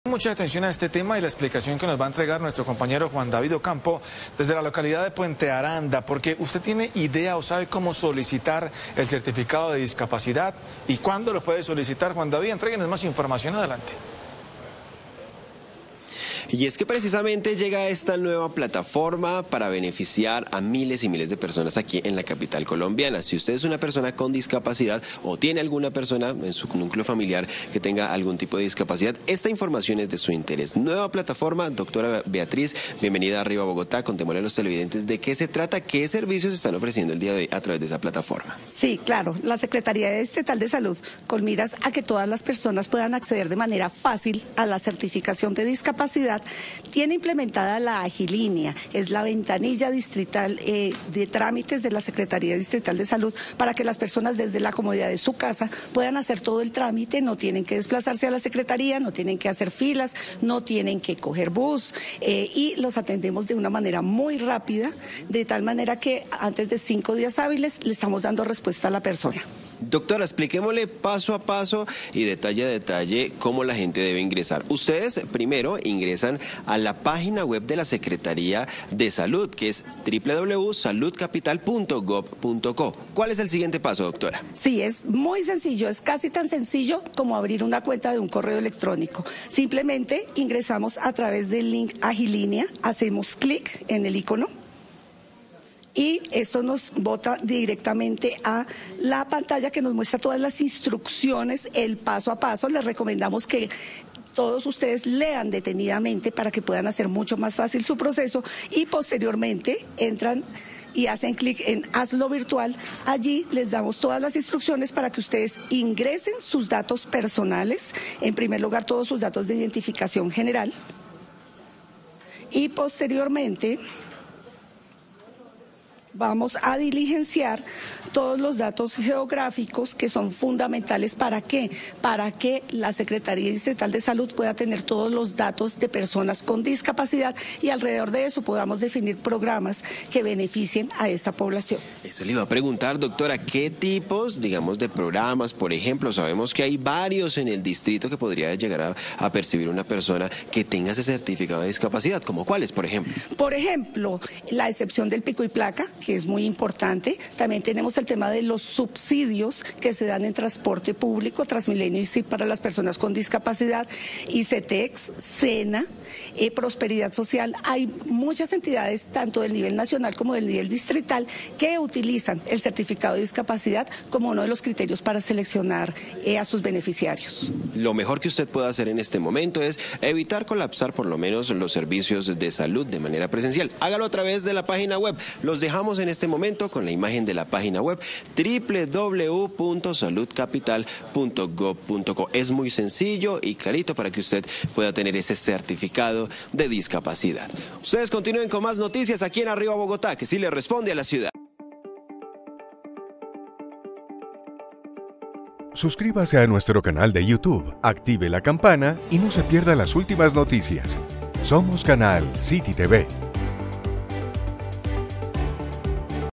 Nueva plataforma para certificado de discapacidad en Bogotá | CityTv
El Distrito y la Secretaría de Salud han lanzado una plataforma de trámites en línea para simplificar el proceso de solicitud de Certificado de Discapacidad en Bogotá. Con cerca de 458,000 personas con discapacidad en la ciudad, esta iniciativa busca agilizar un proceso que antes podía tardar meses o incluso años. Los ciudadanos ahora podrán realizar todo el trámite desde la comodidad de sus hogares.